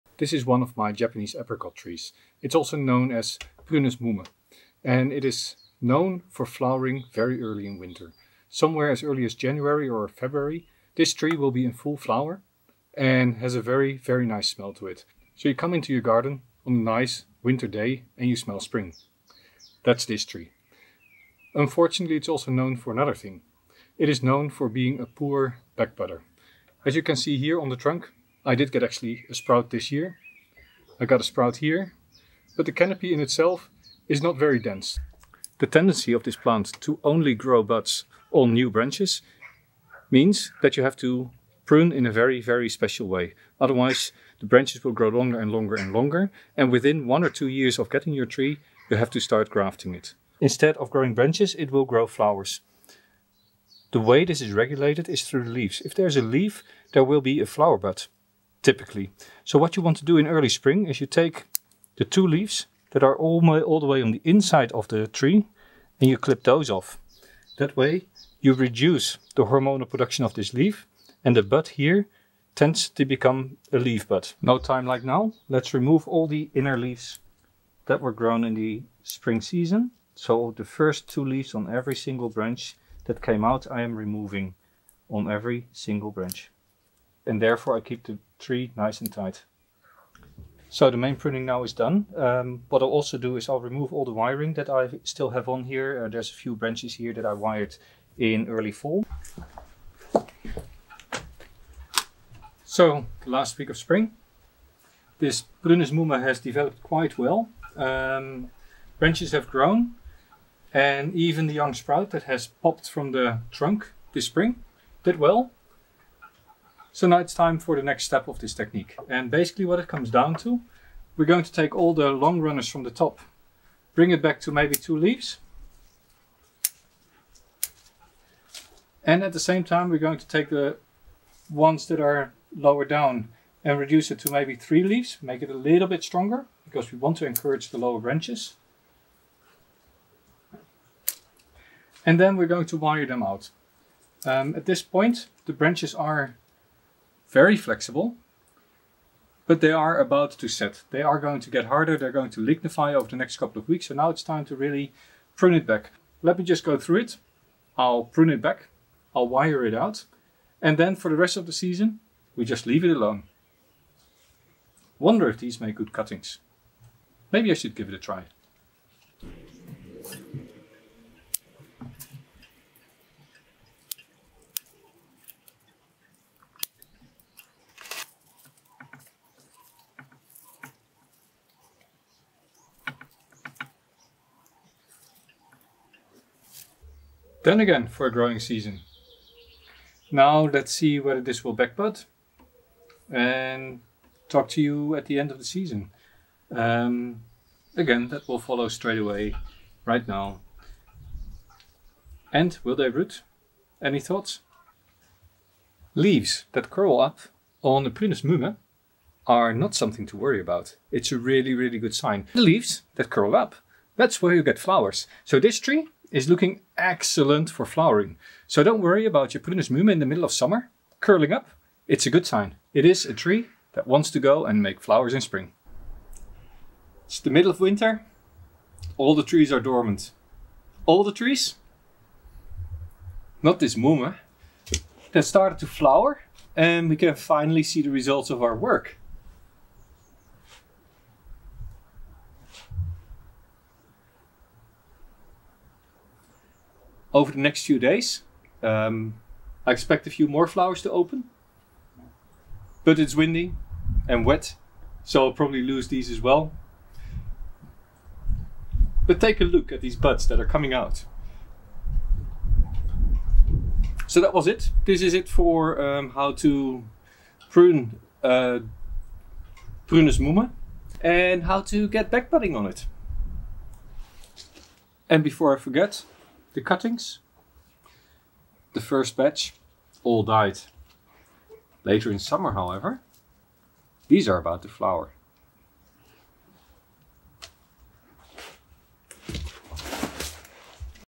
Developing ramification on Prunus Mume (Winter Flowering Japanese Apricot)
Prunus mume is one of the first trees to start flowering in the beginning of the year. With that, it is a great species for bonsai to have in the garden. However, the Japanese apricot also has a tendency to get leggy, with very difficult back budding. Some pruning tips can help to increase back budding and promote better ramification!

0:00 About the Japanese Apricot tree, prunus mume
0.20 Prunus mume and backbudding
0:50 Early spring pruning of leaves prunus mume
1:42 Late spring work: How and why pruning prunus mume for branch budding
3:25 What causes curled up leaves of p mume in summer
3:59 Flowering prunus mume in winter